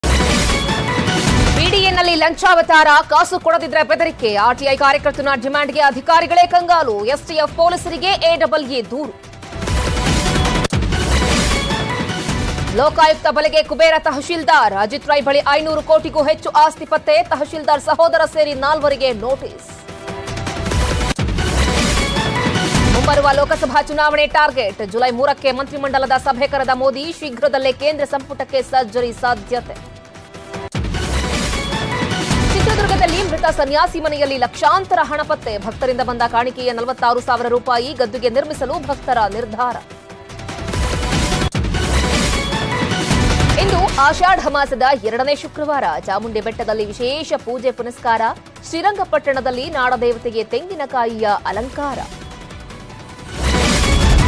10AM Headlines | ಚಿತ್ರದುರ್ಗದಲ್ಲಿ ಮೃತ ಸನ್ಯಾಸಿ ಮನೆಯಲ್ಲಿ ಲಕ್ಷಾಂತರ ಹಣ ಪತ್ತೆ ! | Karnataka Politics
10AM Headlines | ಚಿತ್ರದುರ್ಗದಲ್ಲಿ ಮೃತ ಸನ್ಯಾಸಿ ಮನೆಯಲ್ಲಿ ಲಕ್ಷಾಂತರ ಹಣ ಪತ್ತೆ ! ಭಕ್ತರಿಂದ ಬಂದ ಕಾಣಿಕೆಯೇ 46 ಸಾವಿರ ರೂಪಾಯಿ -ಗದ್ದುಗೆ ನಿರ್ಮಿಸಲು ಭಕ್ತರ ನಿರ್ಧಾರ  | Karnataka Politics